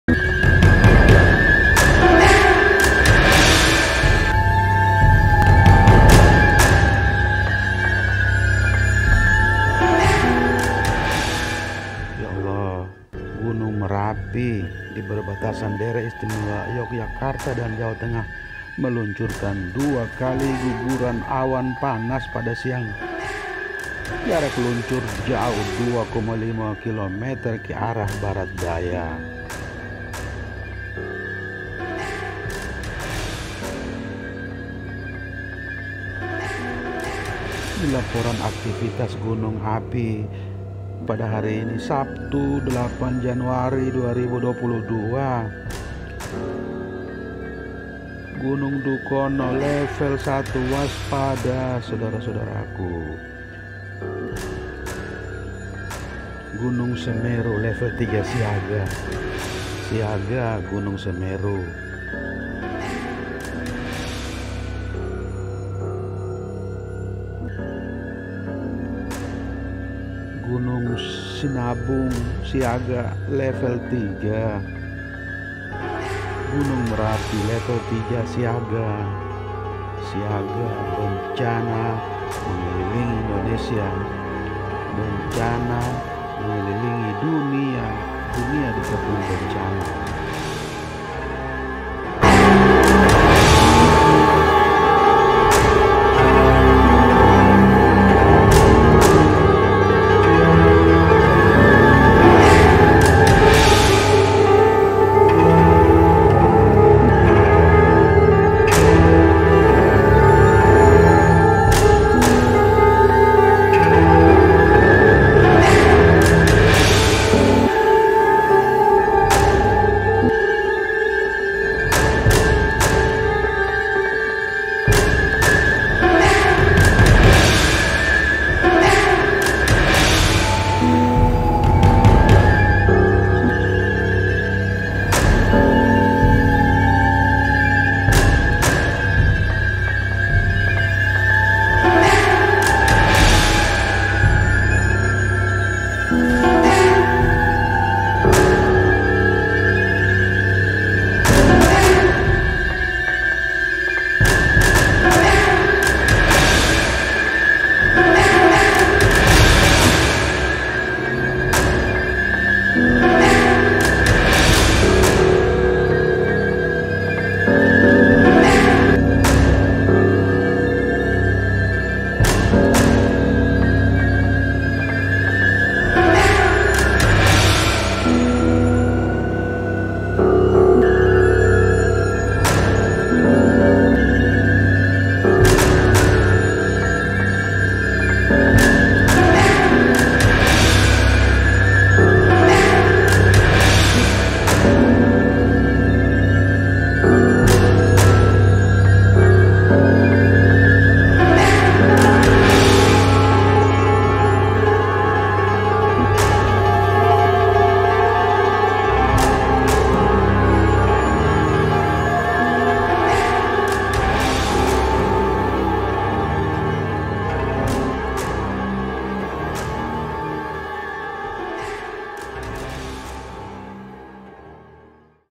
Ya Allah, Gunung Merapi Luncurkan Guguran Awan Panas Dua Kali Hingga 2,5 Km || 10 Gunung Api Waspada
Gunung Merapi Luncurkan Guguran Awan Panas Dua Kali Hingga 2,5 Km.

Gunung Merapi di perbatasan Daerah Istimewa Yogyakarta dan Jawa Tengah meluncurkan dua kali guguran awan panas, pada Jumat siang. Jarak luncur paling jauh 2,5 kilometer ke arah barat daya.

Kepala Balai Penyelidikan dan Pengembangan Teknologi Kebencanaan Geologi (BPPTKG) Hanik Humaida menjelaskan guguran awan panas pertama tercatat pukul 13.29 WIB dengan jarak luncur sejauh 2.500 meter ke arah barat daya.

"Tercatat di seismogram dengan amplitudo 25 mm dan durasi maksimum 223 detik," kata dia dilansir Antara, Jumat (7/1).